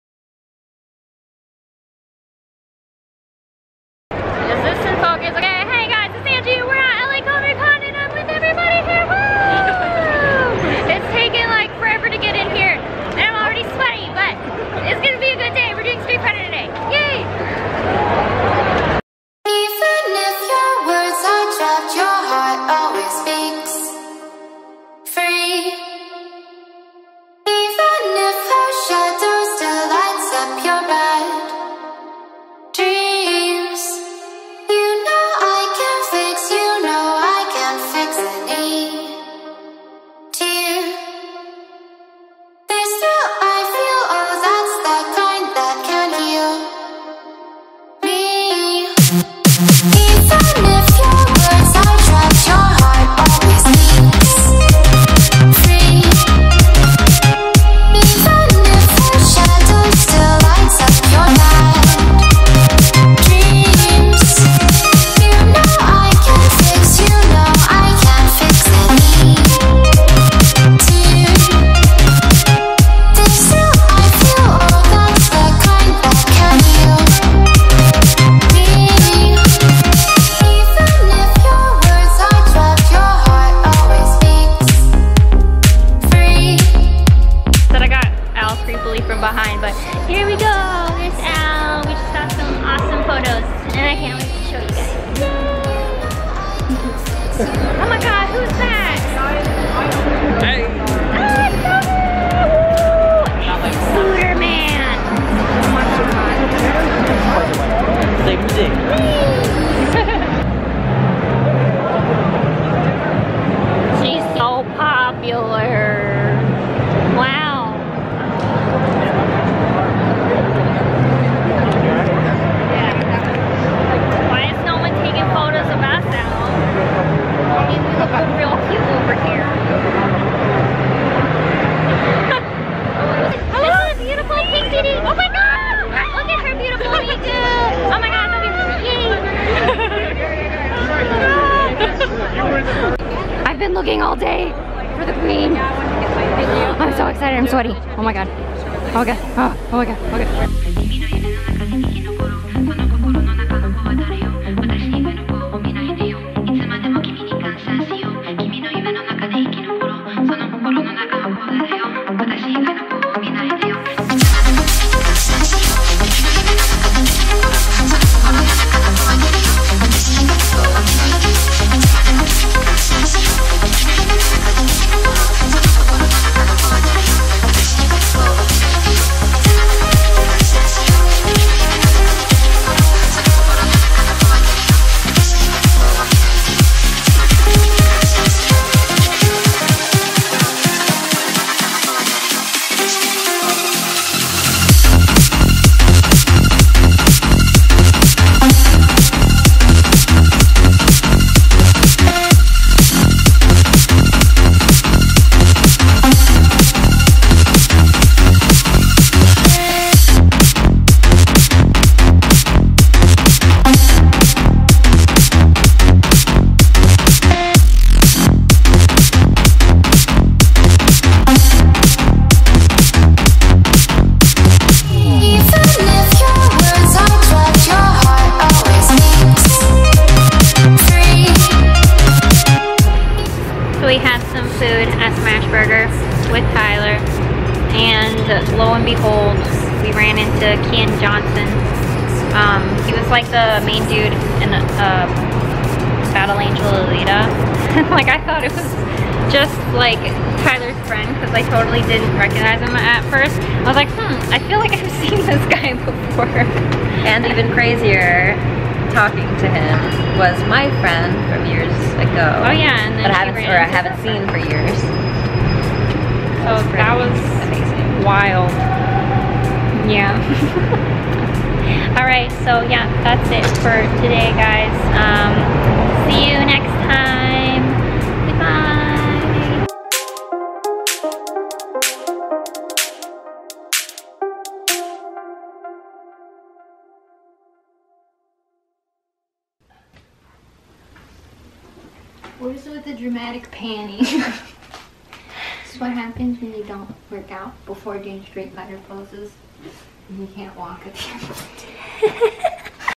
Streetfighter @ LACC 2019 (Saturday) - AnjouPear Cosplay
Hello loves ( ´ ▽ ` )ﾉ
Took my sister to LA Comic Con for her first cosplay and it was a lot of fun!!! It was sooo much more crowded this year so it was harder to see a lot and vlog, so it's mostly just a montage of our shoots at the con~

Thumbnail taken by alkameido on Insta!